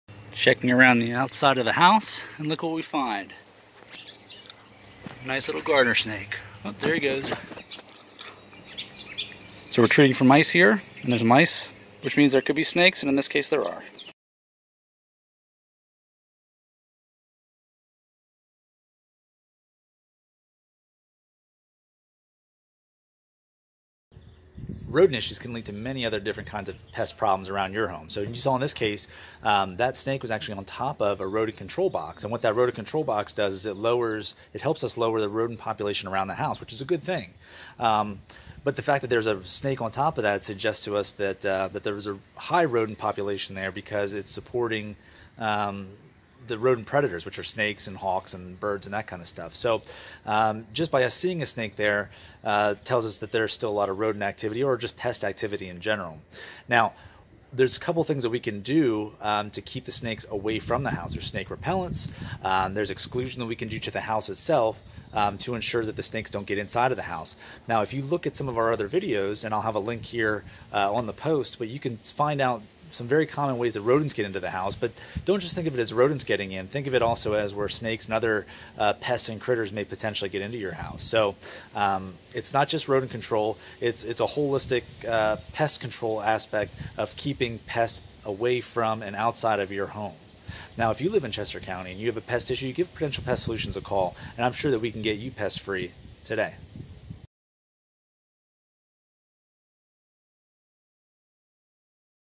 Snakes and Rodents
Found this snake sitting on top of an exterior rodent control station.  The purpose of the station is to reduce the mouse population around this home.  We often find snakes where there is a food source.  Snakes eat many pests and rodents.

Want to keep snakes away from your home?  By eliminating other pests and rodents from inside and around your home, you can help to reduce the chances of having a snake issue.

Do you live in Pennsylvania? Do you want to be pest free?

You can learn more about keeping snakes away from your home by reading our snake prevention tips blog